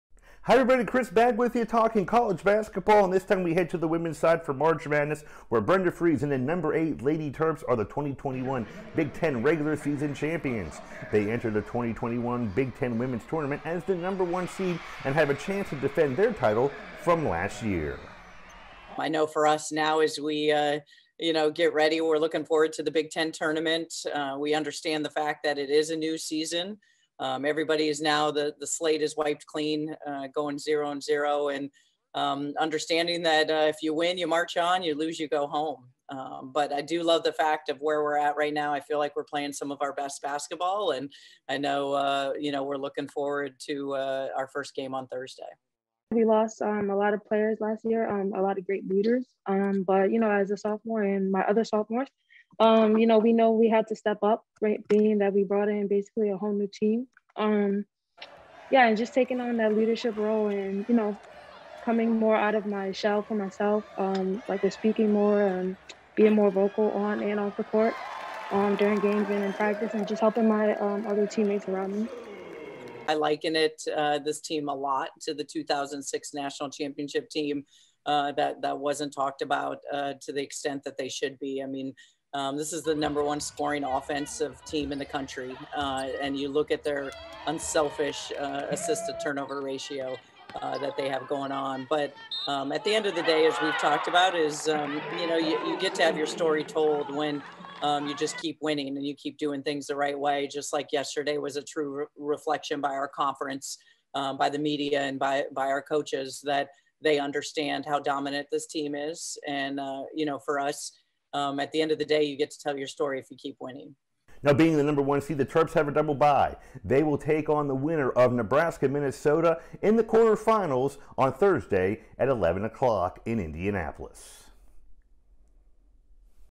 Maryland Lady Terps Big 10 Women's Basketball Tournament preview
The #8 Maryland Lady Terps are the 2021 Big 10 regular season champions and head to Indianapolis looking to win the 2021 Big 10 Women's Basketball tournament and defend their 2020 title.  Head Coach Brenda Frese and Sophomore Guard Ashley Owusu talk about getting set for the tournament.